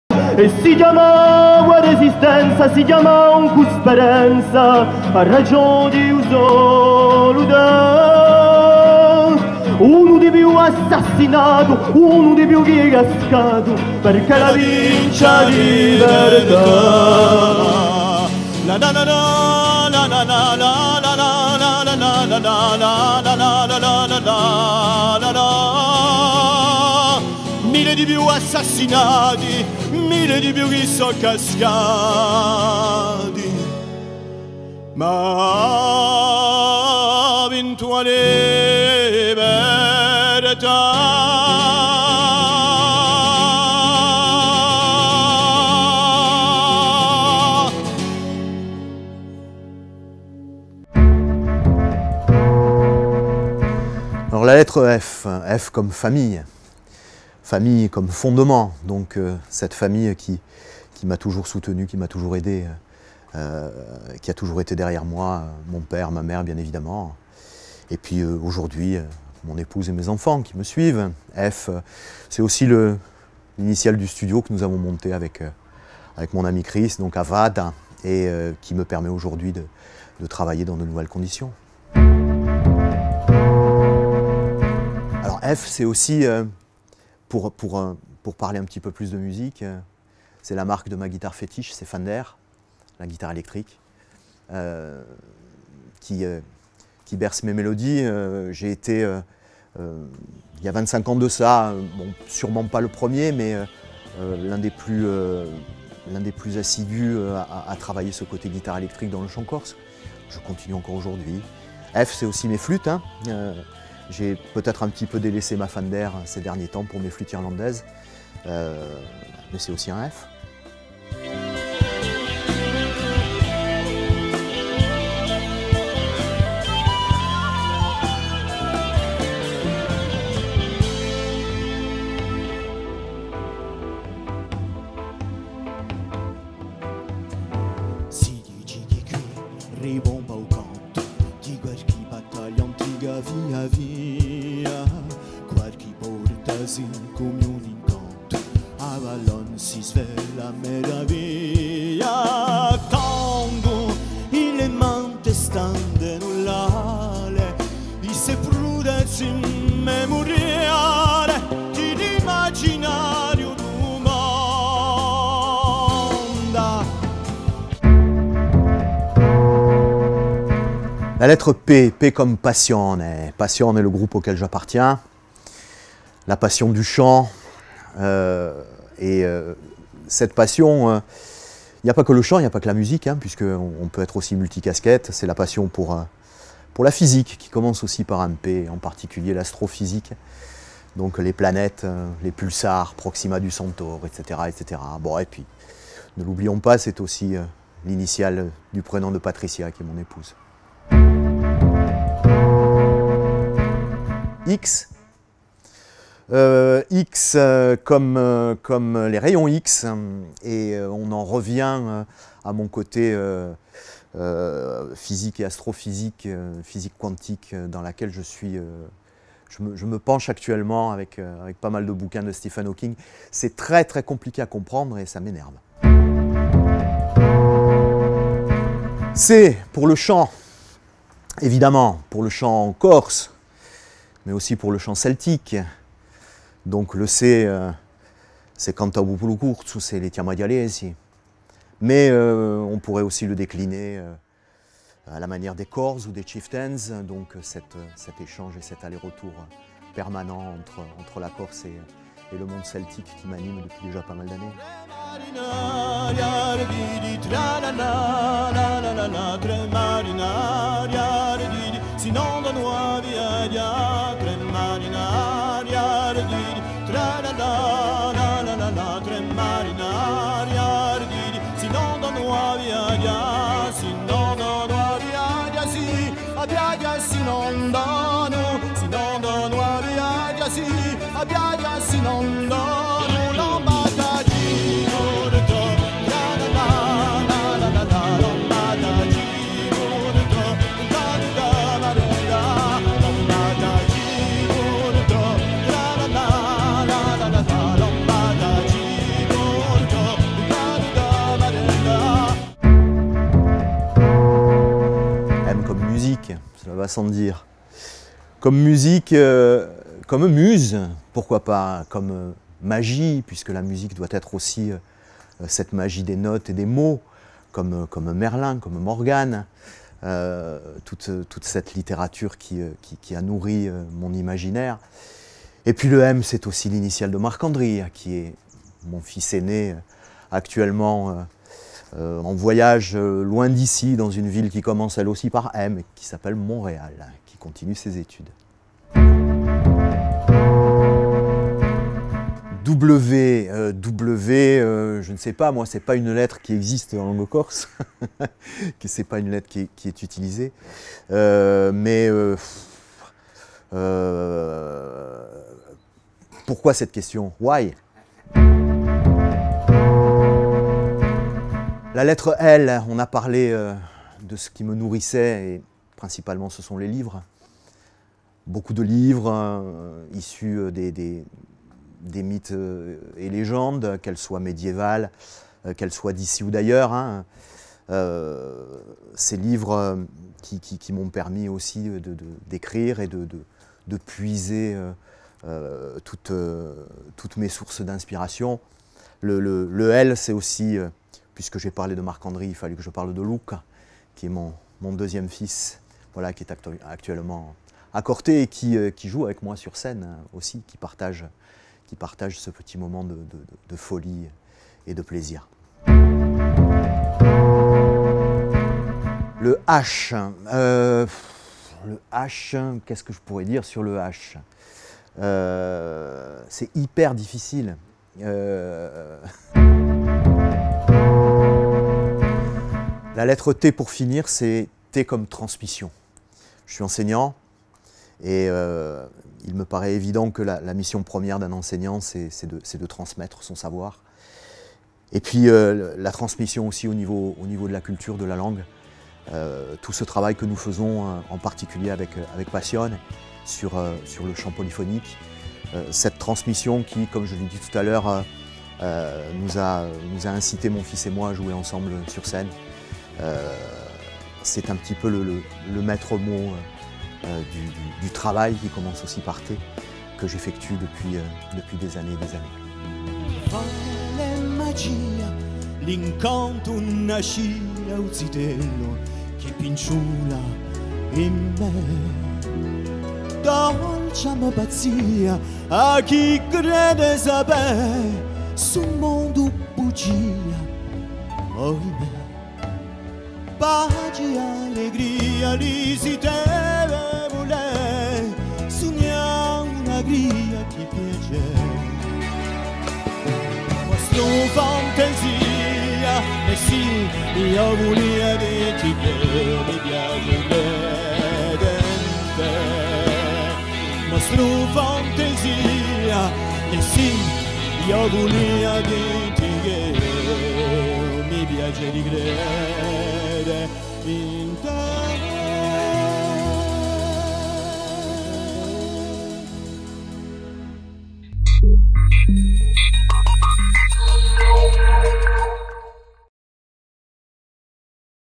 Abécédaire de Jean-Marc Ceccaldi.
Jean-Marc Ceccaldi dévoile son univers artistique dans son abécédaire.